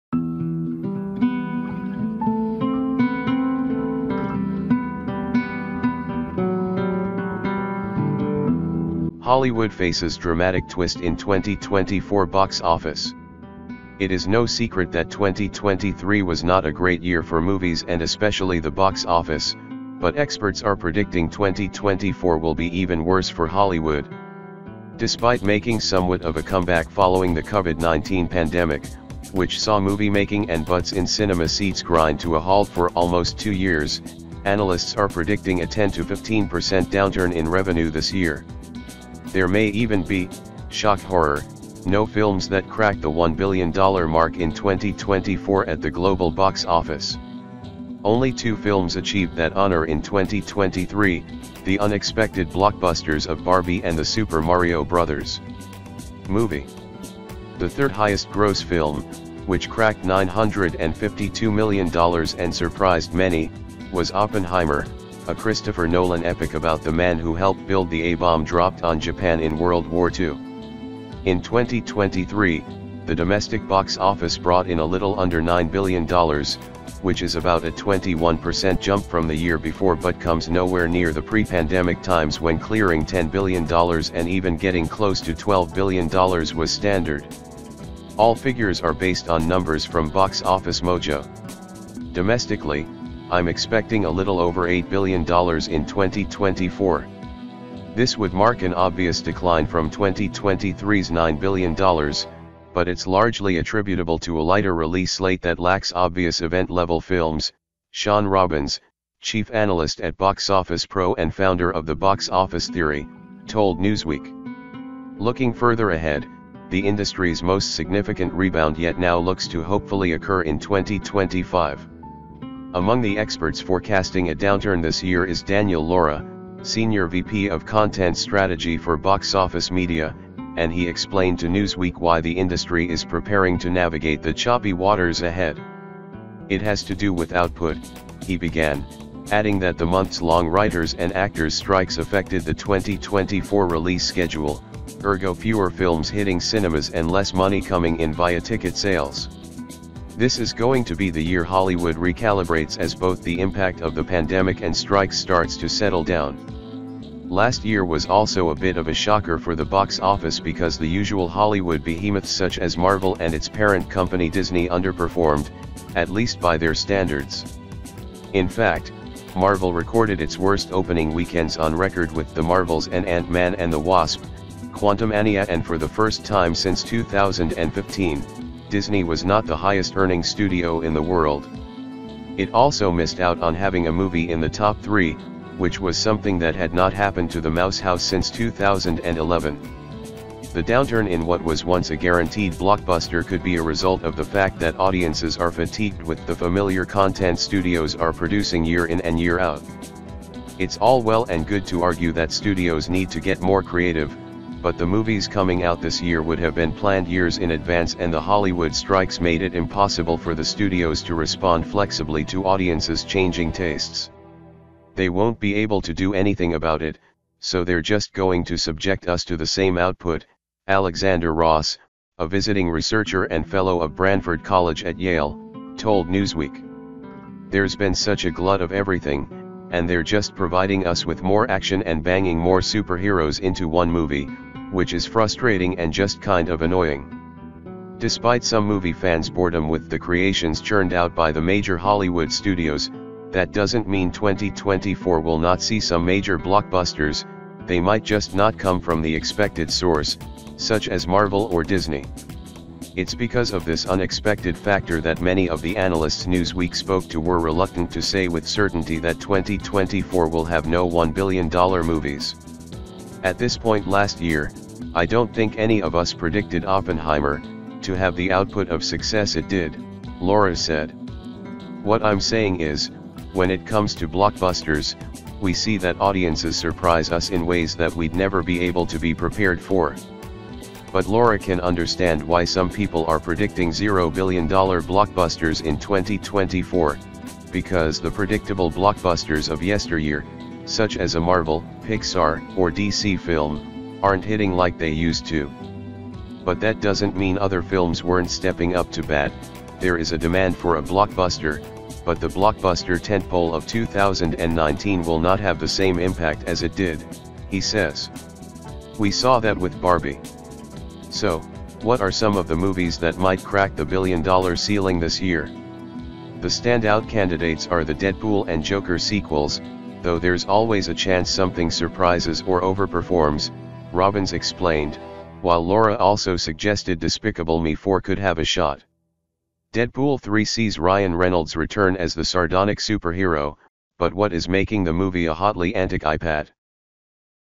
💥Hollywood Faces Dramatic Twist in 2024 Box Office💥
It is no secret that 2023 was not a great year for movies and especially the box office, but experts are predicting 2024 will be even worse for Hollywood.

Despite making somewhat of a comeback following the COVID-19 pandemic, which saw moviemaking and butts in cinema seats grind to a halt for almost two years, analysts are predicting a 10 to 15 percent downturn in revenue this year.

There may even be, shock horror, no films that crack the $1 billion mark in 2024 at the global box office. Only two films achieved that honor in 2023, the unexpected blockbusters of Barbie and The Super Mario Bros. Movie. The third-highest gross film, which cracked $952 million and surprised many, was Oppenheimer, a Christopher Nolan epic about the man who helped build the A-bomb dropped on Japan in World War II.

In 2023, the domestic box office brought in a little under $9 billion, which is about a 21 percent jump from the year before but comes nowhere near the pre-pandemic times when clearing $10 billion and even getting close to $12 billion was standard. All figures are based on numbers from Box Office Mojo.

"Domestically, I'm expecting a little over $8 billion in 2024. This would mark an obvious decline from 2023's $9 billion, but it's largely attributable to a lighter release slate that lacks obvious event-level films," Shawn Robbins, chief analyst at Boxoffice Pro and founder of The Box Office Theory, told Newsweek

"Looking further ahead, the industry's most significant rebound yet now looks to hopefully occur in 2025."

Among the experts forecasting a downturn this year is Daniel Lora, senior VP of content strategy for Boxoffice Media, and he explained to Newsweek why the industry is preparing to navigate the "choppy waters" ahead.

"It has to do with output," he began, adding that the months-long writers' and actors' strikes affected the 2024 release schedule, ergo fewer filmshitting cinemas and less money coming in via ticket sales.

"This is going to be the year Hollywood recalibrates as both the impact of the pandemic and strikes starts to settle down."

Last year was also a bit of a shocker for the box office because the usual Hollywood behemoths such as Marvel and its parent company Disney underperformed, at least by their standards. In fact, Marvel recorded its worst opening weekends on record with The Marvels and Ant-Man and the Wasp: Quantumania and for the first time since 2015, Disney was not the highest-earning studio in the world. It also missed out on having a movie in the top three, which was something that had not happened to the Mouse House since 2011.

The downturn in what was once a guaranteed blockbuster could be a result of the fact that audiences are fatigued with the familiar content studios are producing year in and year out. It's all well and good to argue that studios need to get more creative, but the movies coming out this year would have been planned years in advance and the Hollywood strikes made it impossible for the studios to respond flexibly to audiences' changing tastes.

"They won't be able to do anything about it, so they're just going to subject us to the same output," Alexander Ross, a visiting researcher and fellow of Branford College at Yale, told Newsweek.

"There's been such a glut of everything ... and they're just providing us with more action and banging more superheroes into one movie, which is frustrating and just kind of annoying."

Despite some movie fans' boredom with the creations churned out by the major Hollywood studios, that doesn't mean 2024 will not see some major blockbusters; they might just not come from the expected source, such as Marvel or Disney. It's because of this unexpected factor that many of the analysts Newsweek spoke to were reluctant to say with certainty that 2024 will have no $1 billion movies.

"At this point last year, I don't think any of us predicted Oppenheimer ... to have the output of success it did," Lora said. "What I'm saying is, when it comes to blockbusters, we see that audiences surprise us in ways that we'd never be able to be prepared for."

But Lora can understand why some people are predicting zero billion-dollar blockbusters in 2024, because "the predictable blockbusters of yesteryear," such as a Marvel, Pixar or DC film, aren't hitting like they used to.

"But that doesn't mean other films weren't stepping up to bat ... there is a demand for a blockbuster, but the blockbuster tentpole of 2019 will not have the same impact as it did," he says. "We saw that with Barbie."

So, what are some of the movies that might crack the billion-dollar ceiling this year?

"The standout candidates are the Deadpool and Joker sequels, though there's always a chance something surprises or over-performs," Robbins explained, while Lora also suggested Despicable Me 4 could have a shot.

Deadpool 3 sees Ryan Reynolds return as the sardonic superhero, but what is making the movie a hotly anticipat